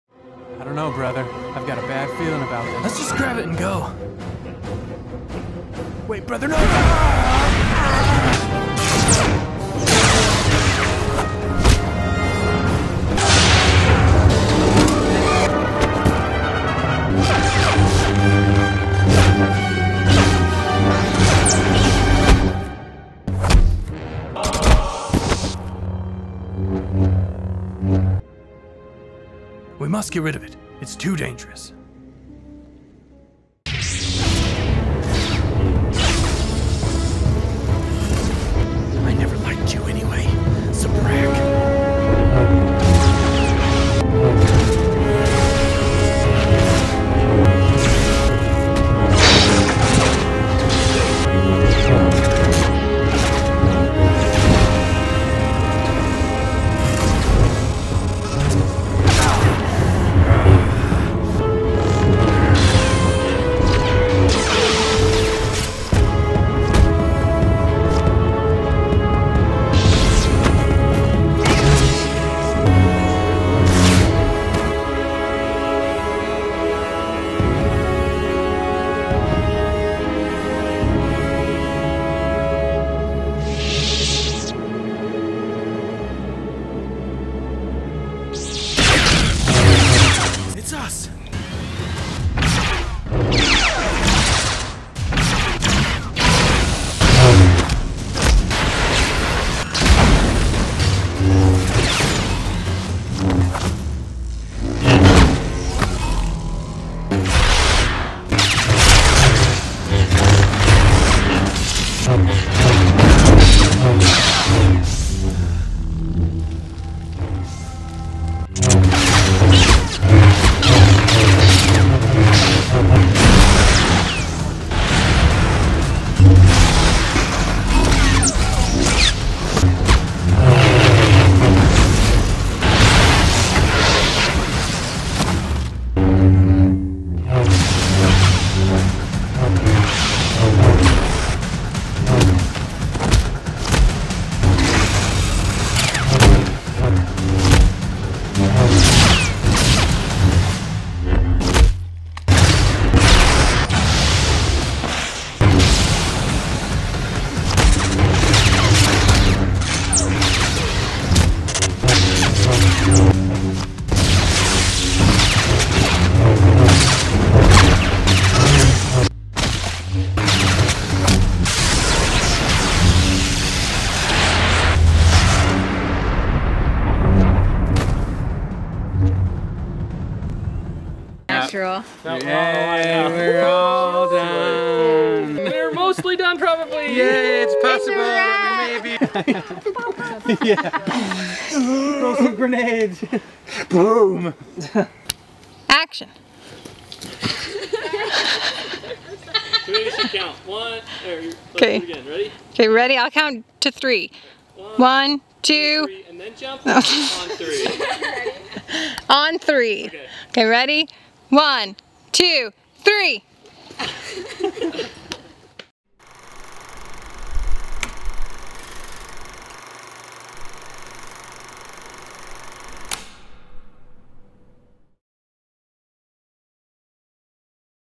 Hawkstudio Productions "Greatest Hits" - Star Wars Fan Film Highlights
Here is a "Greatest Hits" highlight reel of our 3 Star Wars short films.  And by "Hits" we mean impacts like lightsabers clashing, blasters blasting, fists and elbows flying and all that good stuff!  Enjoy!